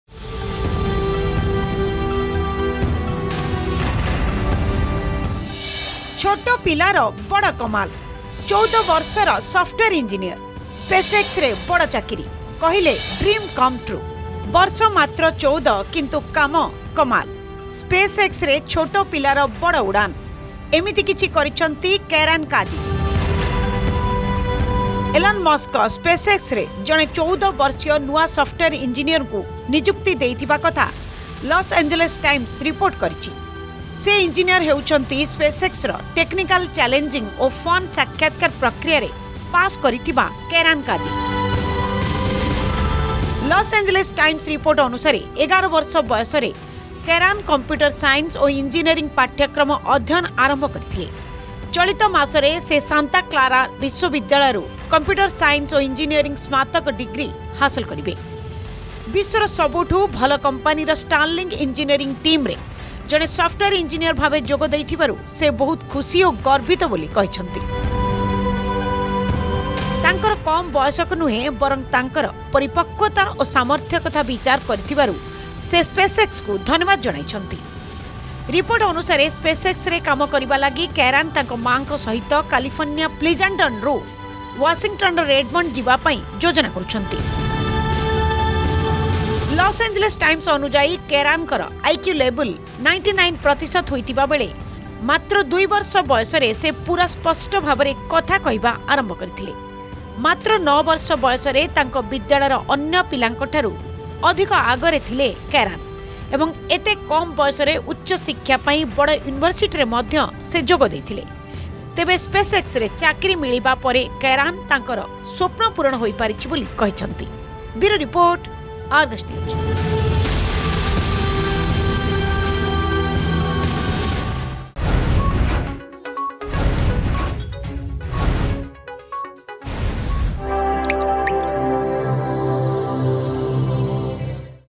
14 Years Old Kairan Quazi Proves Age Is Just A Number, Joins SpaceX As Software Engineer
14 Years Old Kairan Quazi Proves Age Is Just A Number, Joins SpaceX As Software Engineer.